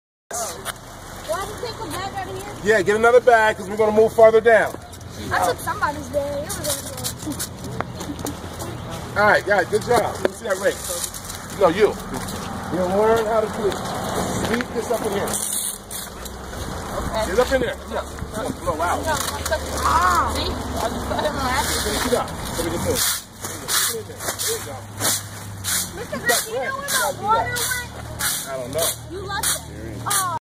Cross Ages clean up
Dejuann Greene, Youth Program Specialist at Tri-County Community Action, instructs boys in the Cross Ages mentoring program as they pick up trash along Derry Street as part of Saturday&apos;s city-wide clean-up. Greene said the program is always loo